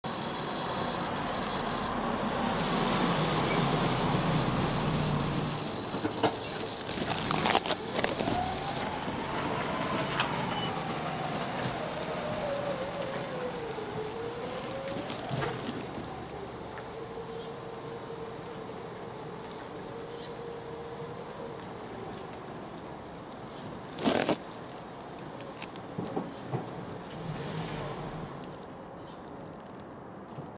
空から謎の落下物ファフロツキーズFalls From The Skies/ FAFROTSKIES
寒い冬の早朝、みぞれが降ってきた。しばらくして外に出ると、道路一面に透明で直径１ｃｍくらいの球体がたくさん落ちていた。これは、ファフロツキーズなのか？同じような現象が世界中で起きているという。In a cold winter early morning, sleet fell. A lot of globes of around 1cm in diameter fell with transparence in a few minutes all over the road when I went out. It is said that a similar?phenomenon is up all over the world whether this is FAFROTSKIES.